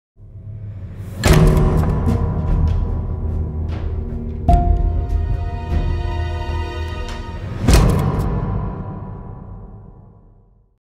Ο Ορφέας Αυγουστίδης στην παράσταση "7 χρόνια" στο θέατρο Αποθήκη.
7 ΧΡΟΝΙΑ
Σκηνοθεσία: Οδυσσέας Παπασπηλιόπουλος
Από τις 18 Οκτωβρίου